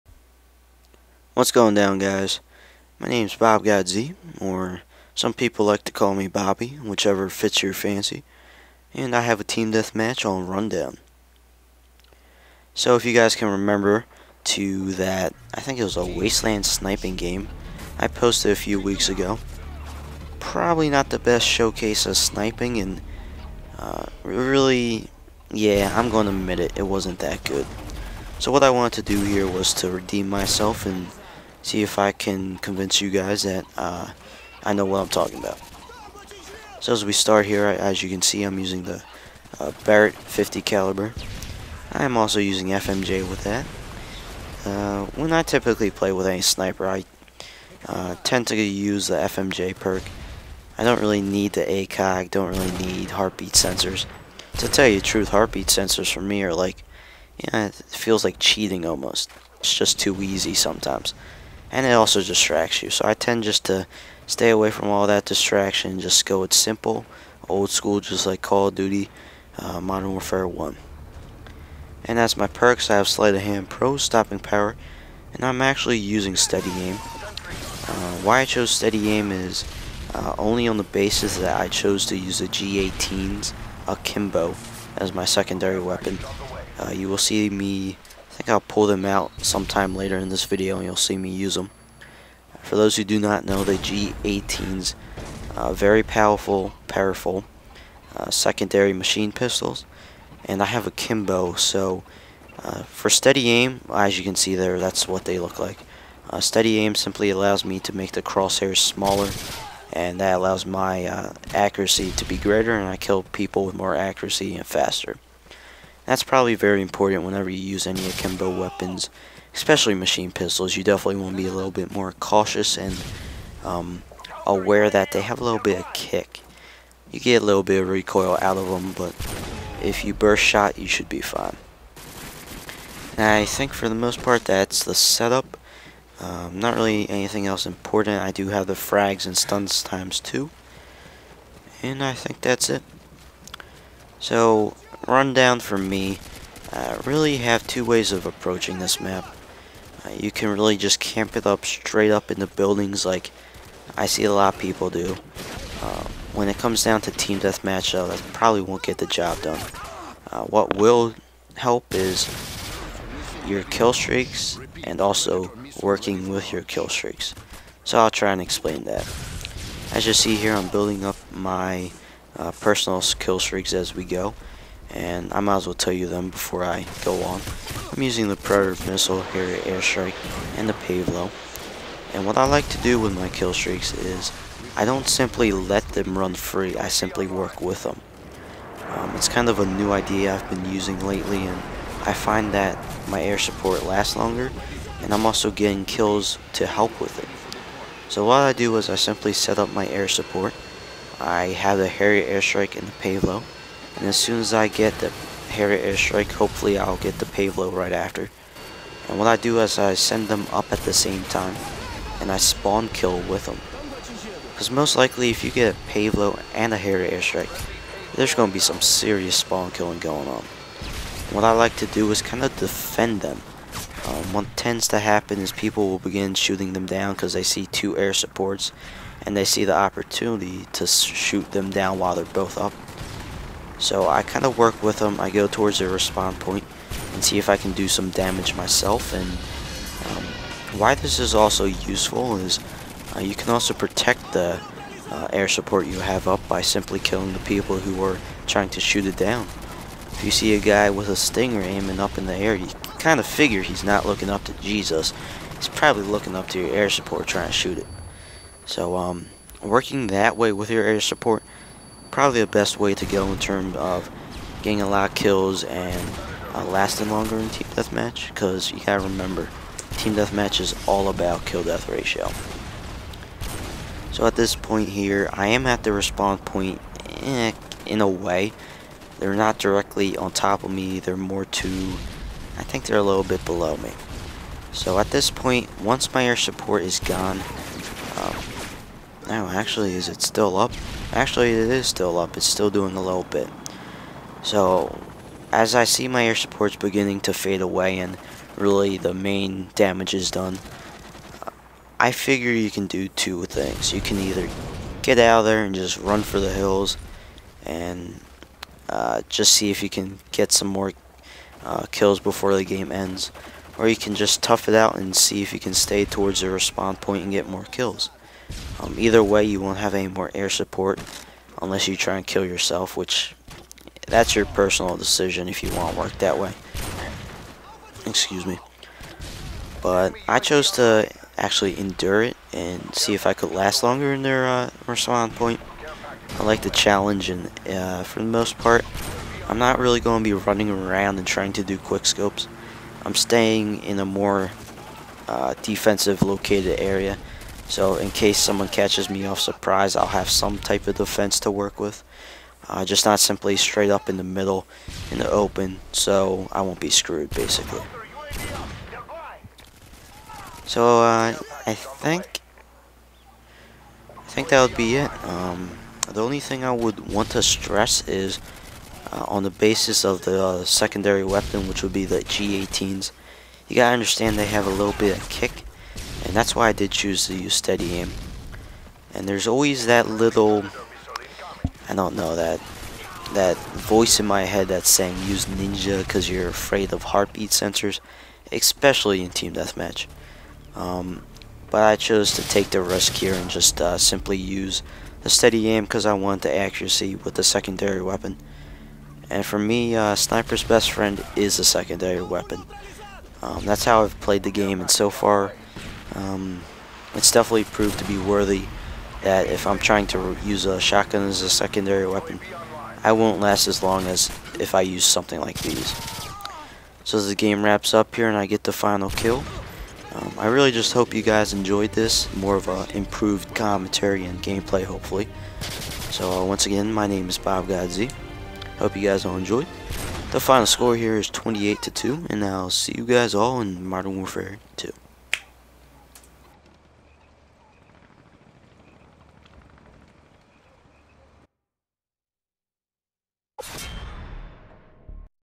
GameZoneGlobal: BobGodz: Team Deathmatch #1
Ranked Match: BobGodz: Team Deathmatch: Rundown

Gameplay: BobGodz
Commentary: BobGodz

This is his second video on GzG.